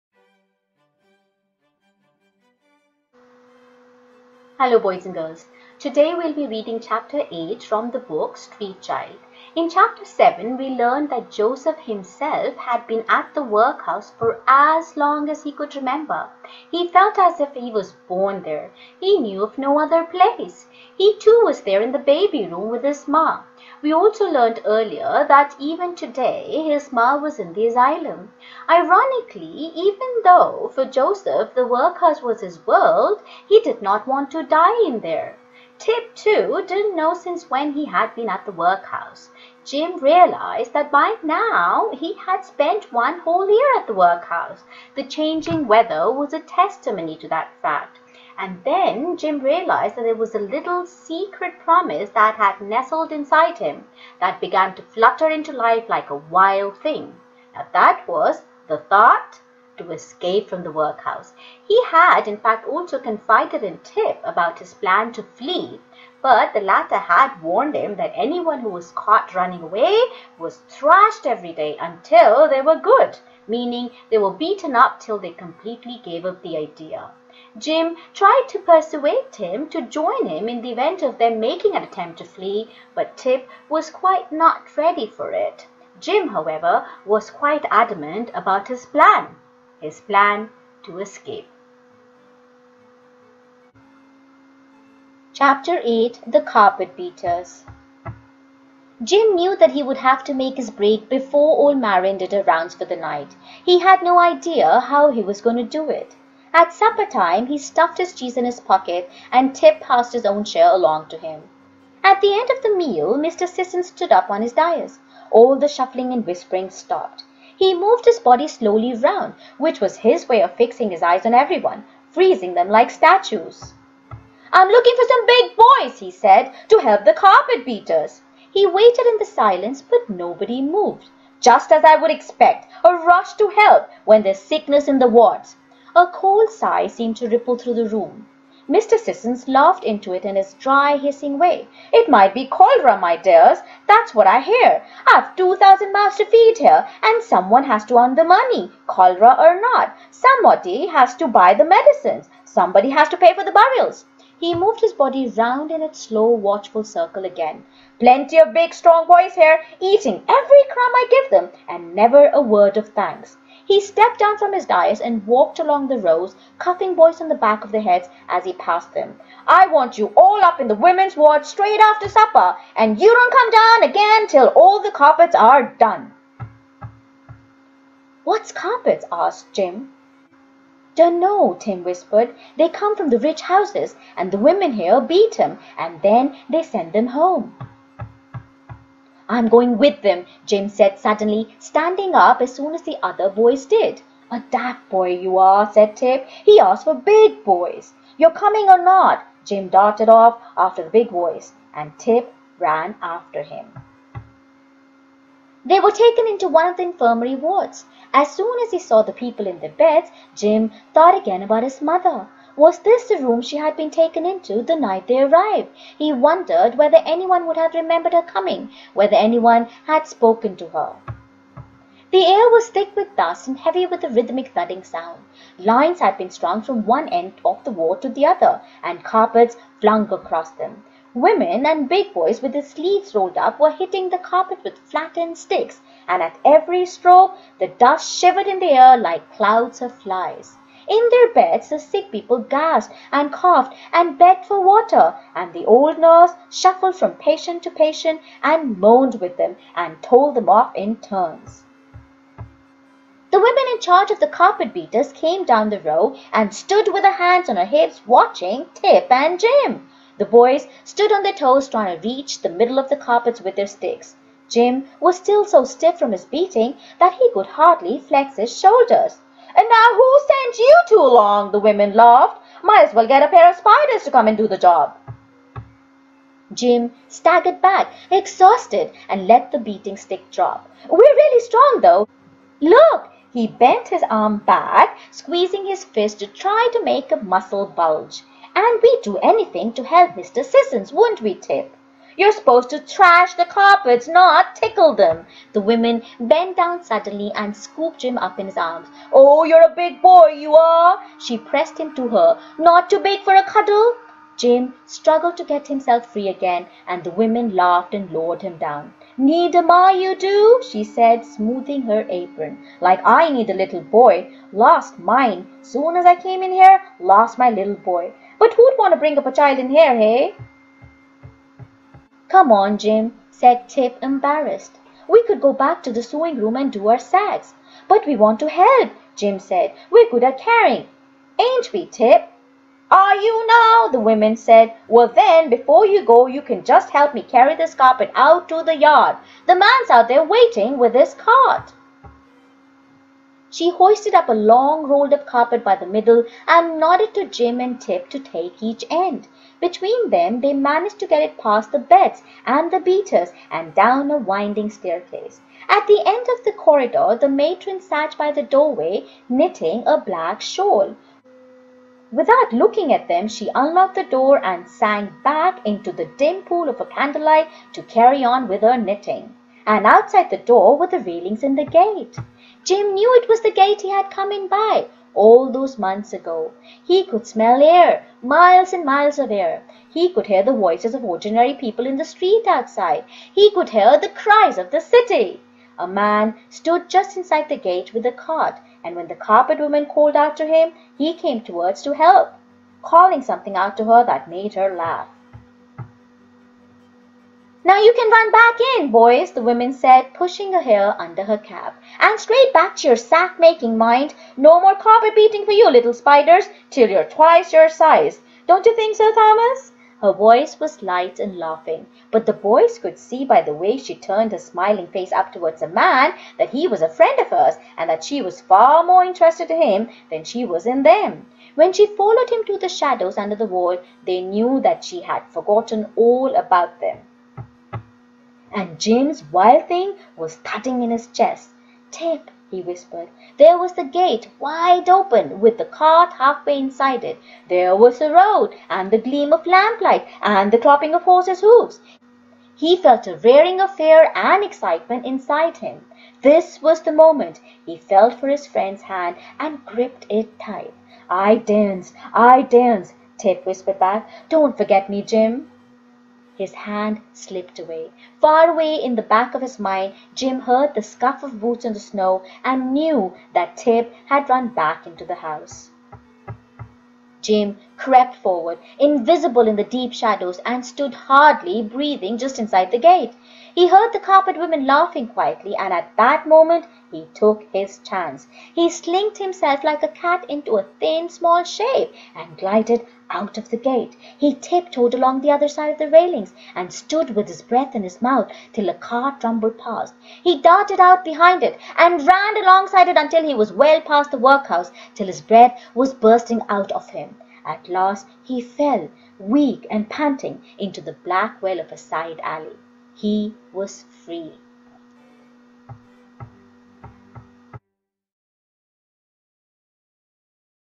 CHAPTER 8 - STREET CHILD by Berlie Doherty
Read by Mona Sharma Gupta
Summary of Chapter 7

In Chapter 7, we learnt that Joseph himself had been at the workhouse for as long as he could remember. He felt as if he was born there. He knew of no other place. He too was there in the baby room with his Ma. We earlier learnt that even today his Ma was in the asylum. Ironically, even though for Joseph the workhouse was his world, he didn’t want to die in there. Tip too, didn’t know since when he had been in the workhouse. Jim realized that by now he had spent one whole year. The changing weather was a testimony to that.  And then Jim realized that there was a little secret promise that has nestled inside him, that began to flutter into life like a wild thing. That was the thought to escape from the workhouse. He had confided in Tip about his plan to flee. The latter had warned him that anyone who was caught running away was thrashed every day until they were good meaning they were beaten up till they completely gave up the idea. Jim tried to persuade Tip to join him in the event of them making an attempt to flee but Tip was just not ready. Jim, however was quite adamant about his plan. His plan to escape.

Check out the sing along book - Tender Times by Mona Sharma Gupta, which has a collection of 60 songs/ rhymes and short phonic stories.